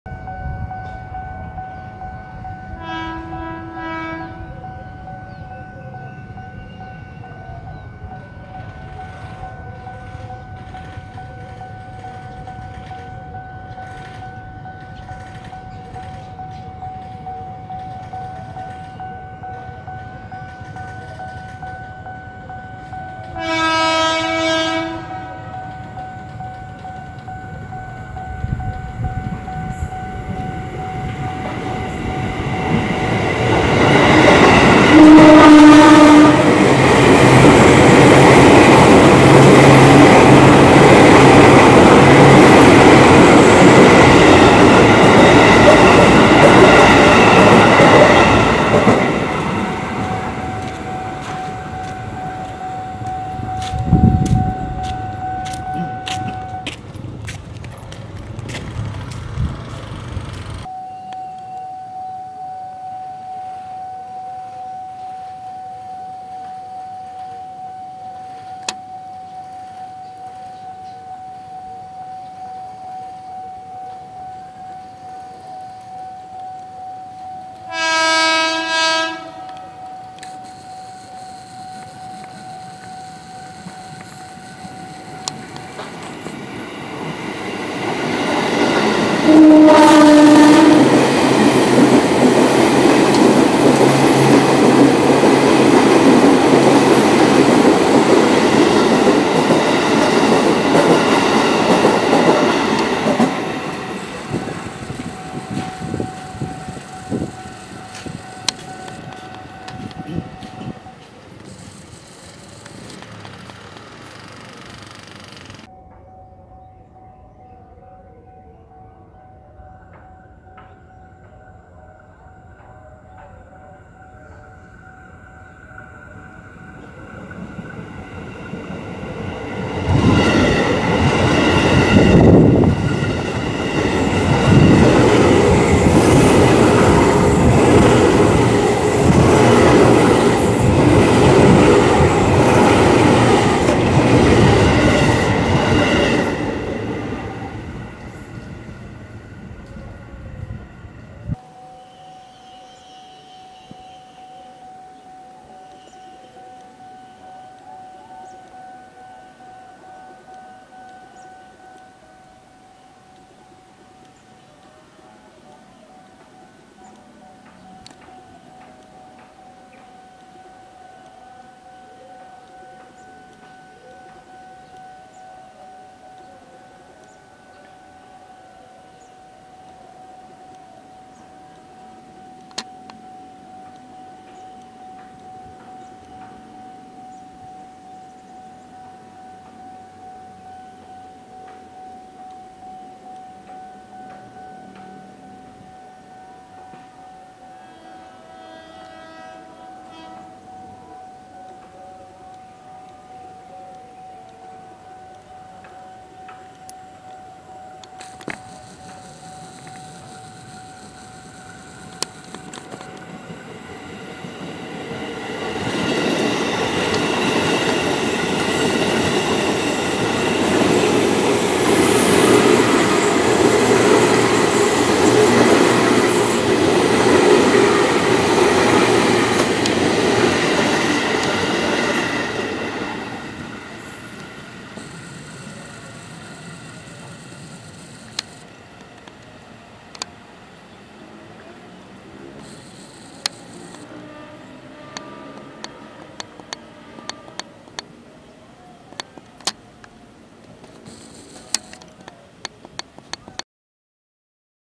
Stasiun Kereta Api Non Aktif
Stasiun Kereta Api Non Aktif Pondok Rajeg, adalah salah satu stasiun yang terletak di Daerah Operasi  1 Jakarta. Lebih tepatnya terletak di Pondok Rajeg, Cibinong, Bogor. Stasiun ini dibangun antara tahun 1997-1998. Stasiun ini sempat beroperasi dari tahun 1999 sampai tahun 2006 . Kereta yang beroperasi pada saat itu adalah Kereta Rel Diesel (KRD) Nambo. Pada tahun 2006, KRD Tersebut sudah tak layak lagi beroperasi dan akhirnya  jalur ini pun ikut dinonaktifkan.